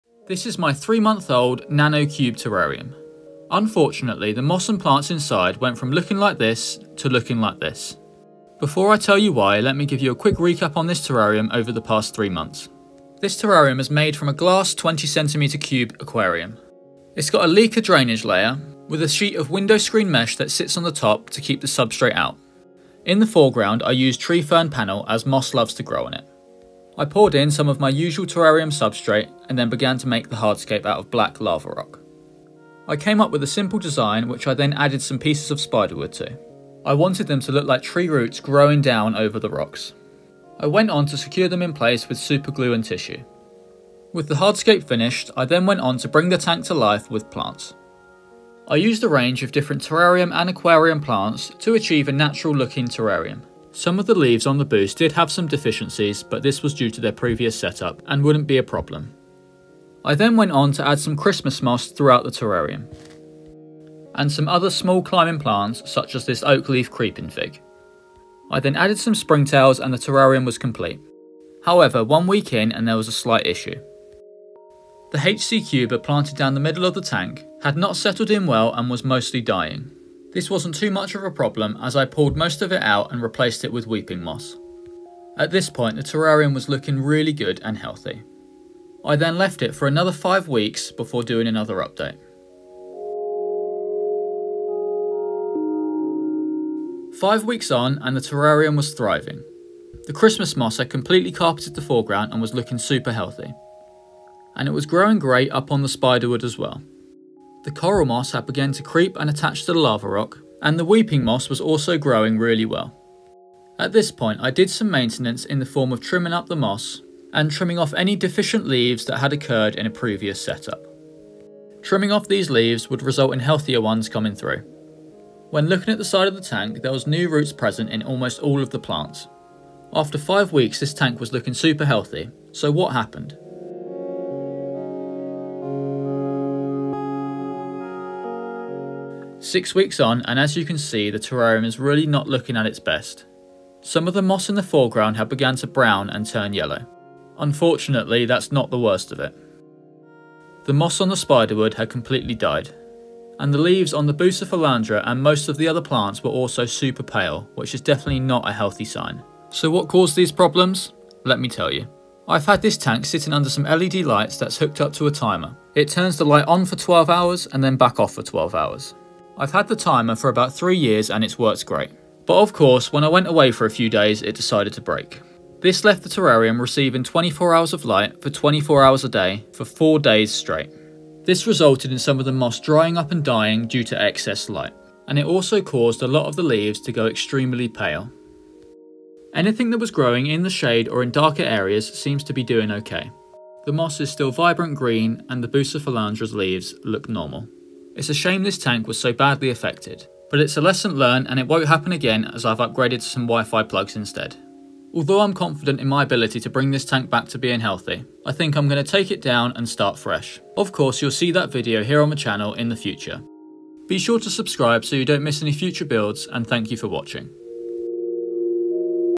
This Wasn’t Supposed To Happen…
In this video I’ll be sharing the unfortunate problem I had with this nano cube terrarium. It’s definitely looking far from its best… hope you enjoy the video & don’t make this mistake yourself!

Plants:
Begonia vankerckhovenii
Bucephalandra sp
cryptocoryne parva
Anubias Nana
Christmas moss (Vesicularia montagnei)
Coral moss (Riccardia chamedryfolia)
HC Cuba (Micranthemum callitrichoides)
Weeping moss (Vesicularia ferriei)
Oak leaf creeping fig (ficus quercifolia)
Marcgravia small round

Substrate:
Coco fibre
Sphagnum moss
Orchid bark
Worm castings
Aqua soil

20 x20 tank
Leca drainage
Window screen mesh
Tree fern fibre
Spider wood
Good LED light
Terrarium tools
Continuous misting bottle
Rack Lighting

Substrate:
Coco fibre
Sphagnum moss
Orchid bark
Worm castings

20 x 20 Tank
Window screen mesh
Tree fern fibre
Good led light
Terrarium tools
Spider wood
Lava rock
Continuous misting bottle
Rack lighting